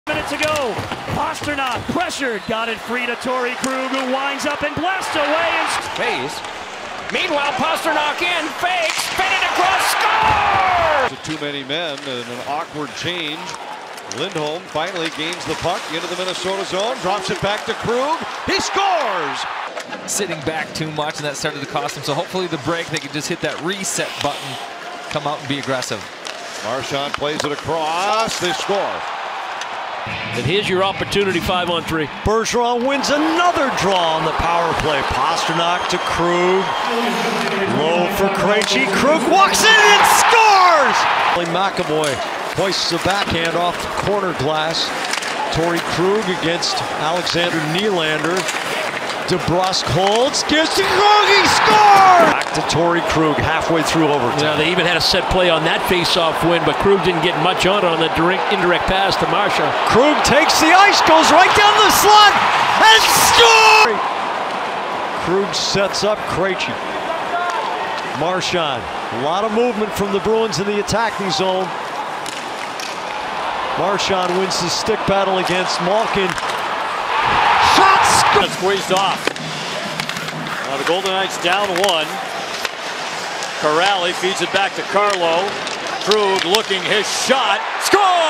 Torey Krug Highlight
GOOD night stlblues fans I just made another video for you with your new player from the UFA, Torey Krug. Watch is 9 goals from last season. Great signature !!